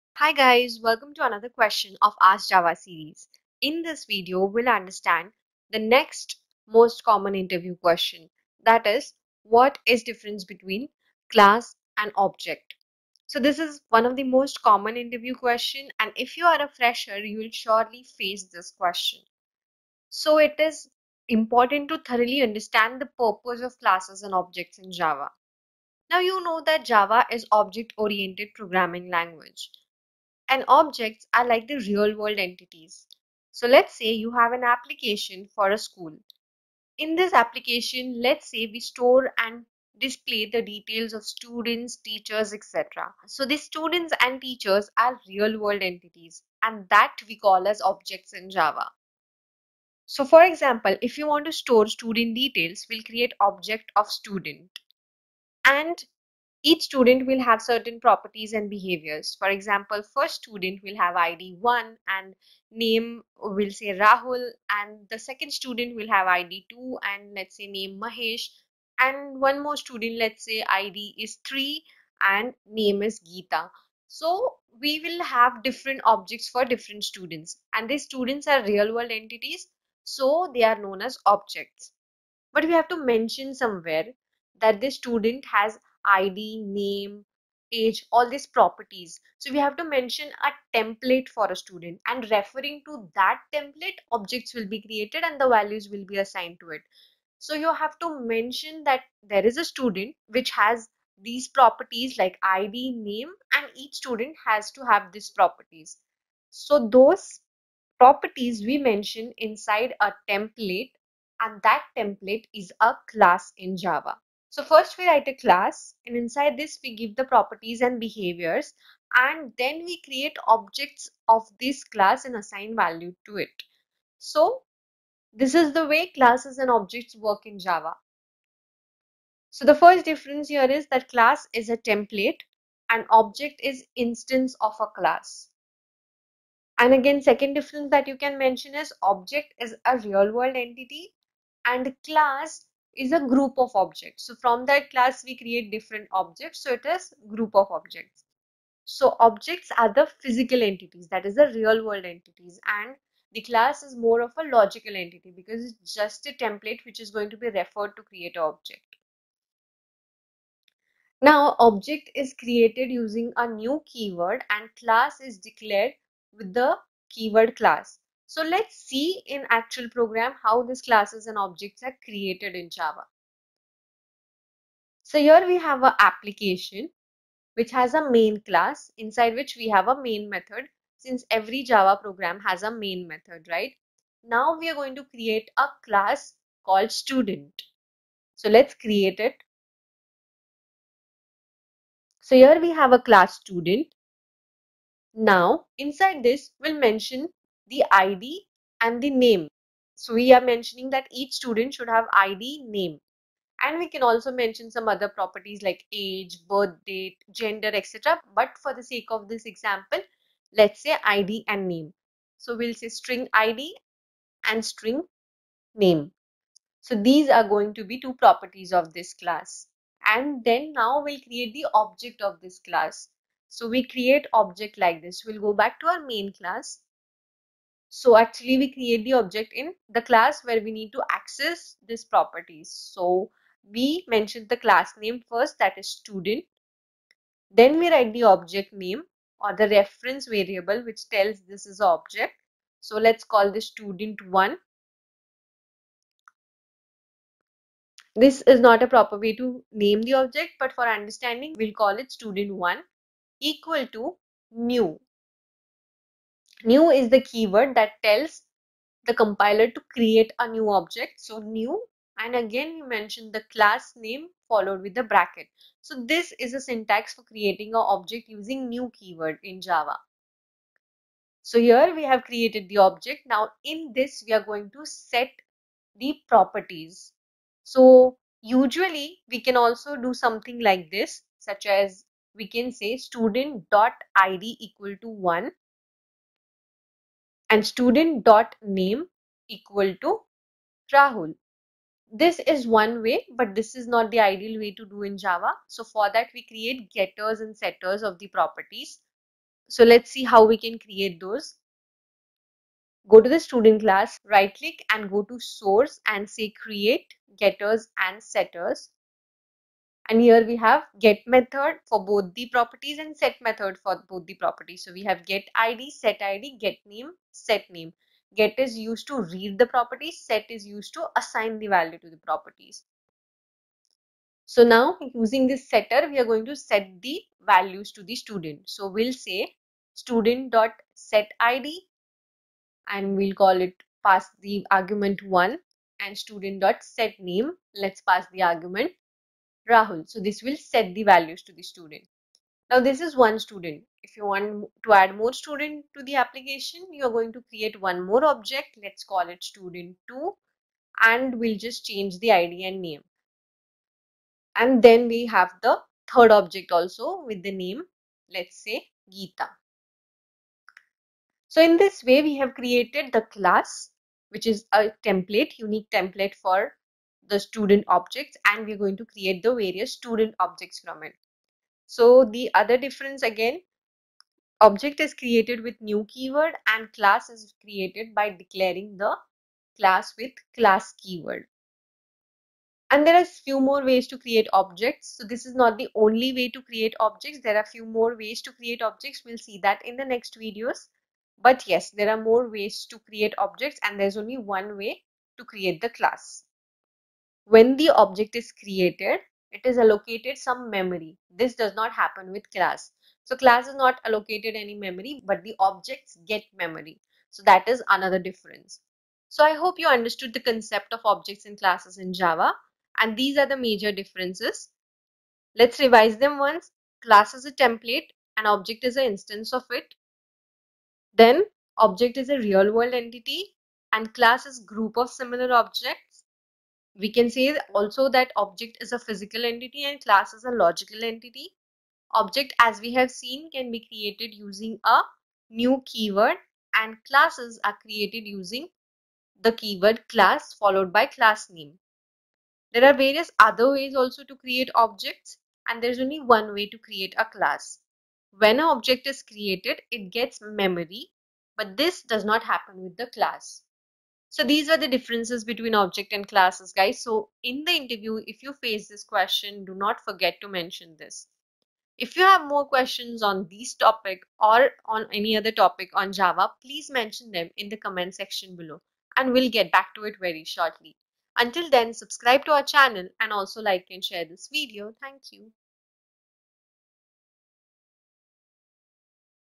What is the difference between class and object | Java Interview Question | Ask Java | Code Bode
Hi Guyz, In this video, we will understand the difference between class and object in java

HAPPY LEARNING!